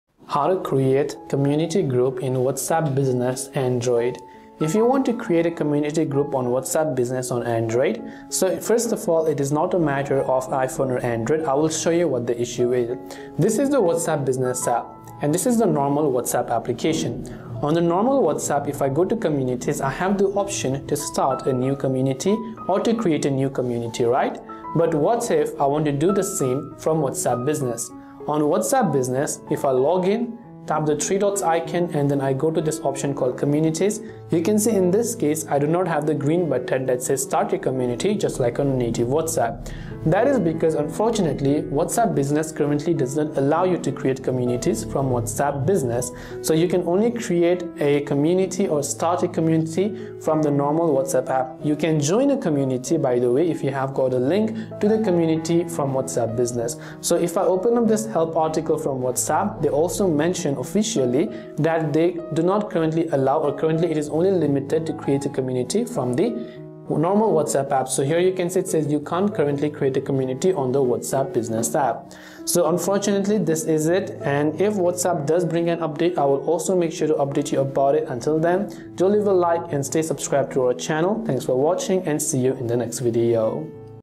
How to Create Community Group in WhatsApp Business Android !
➜ This video explores how to create a community group in WhatsApp Business on an Android device and clarifies why this feature currently remains unavailable. At the start, the presenter points out that the limitation has nothing to do with whether you are using Android or iOS. Instead, it is related to restrictions within the WhatsApp Business app itself. The regular WhatsApp app provides a clear option to create or start new communities, while WhatsApp Business only allows users to join existing communities via invitation links. This distinction often confuses business owners and organizations who are already benefiting from the specialized features of WhatsApp Business, such as auto-replies and detailed business profiles, because they naturally expect these features to also include the flexibility to create communities for marketing or group announcements.

Communities, as implemented on the regular WhatsApp app, allow administrators to group multiple sub-chats together under one overarching umbrella. For example, a teacher could place various class groups under a single community, or a neighborhood association could contain multiple interest groups under the same digital space. Unfortunately, for those who rely on WhatsApp Business, creating such communities remains impossible as of the recording of this video. Some might wonder whether the presenter’s device or operating system type affects the feature, but the limitation is confirmed to be app-based. The official WhatsApp help center also documents that WhatsApp Business does not yet support community creation, meaning the problem persists no matter where you install it.

The presenter walks viewers through the steps one might take if creating communities were allowed on WhatsApp Business. You would typically tap the three dots on the top right to open the menu, select the Communities option, and then look for a “Start Community” or “Create Community” button. In the regular WhatsApp, this button is clearly visible at the top or bottom of the Communities page. In WhatsApp Business, however, the feature is conspicuously absent. Instead, the app only offers the ability to view or join existing communities, indicating that the app developers might be testing or refining the feature exclusively in the standard version before eventually rolling it out to WhatsApp Business.

The video concludes by confirming that even official WhatsApp documentation states users of WhatsApp Business “can’t currently create a community,” which suggests it may still be in development or under consideration for a future update. The presenter advises that there is no confirmed timeline for when or if community creation will be added, but staying updated with app releases might eventually yield good news. Those who truly need community creation today might be forced to switch to the standard WhatsApp app for that specific function, then switch back to WhatsApp Business for their day-to-day business interactions. This split usage is inconvenient, but for now it is the only workaround for those who rely heavily on community-based group management features while also wanting the specialized tools of WhatsApp Business.

For many, the question remains: will WhatsApp unify its offerings and allow business accounts to create communities soon? It makes sense for corporate and entrepreneurial users to have access to the same robust group management options that personal users already enjoy. WhatsApp might be analyzing the potential impacts or ensuring that advanced business features do not overwhelm small business owners who prefer simpler functionality. Only time will tell whether a future update will bring these features into a single convenient ecosystem. In the meantime, the best you can do is join existing communities from within WhatsApp Business or switch temporarily to the standard app if you want to start one on your own. Subscribing to this channel and staying informed on the latest WhatsApp changes is the recommended approach to remain in the loop regarding any community features that might land in WhatsApp Business in the future.

0:00 Introduction
0:02 Checking WhatsApp Business
0:09 Comparison with Regular WhatsApp
0:19 Communities Feature Explained
0:26 No Create Option on Business App
0:45 Official WhatsApp Documentation
1:00 Conclusion